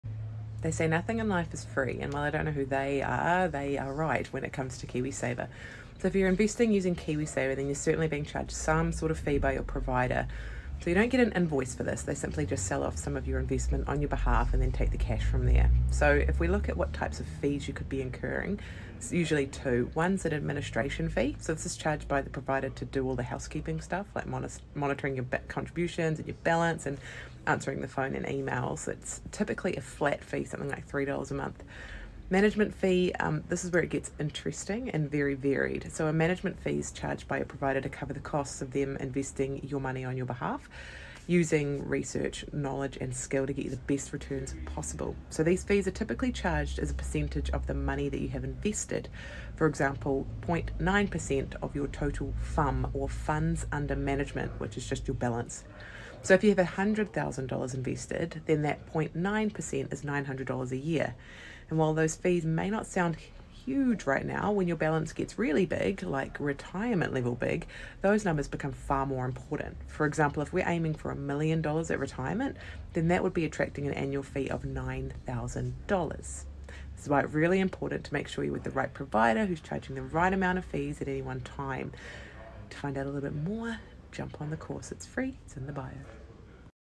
Why Do We Have to Pay Fees?
Fees can be a real pain point, especially when they're pocketed by the finance industry.

But it's worth asking - what value do we actually get for the price we pay for KiwiSaver.

Over time, these fees can add up to a hefty sum. It's time to understand and assess if they're truly worth it. Let's dive in and unravel the cost-benefit analysis of financial fees.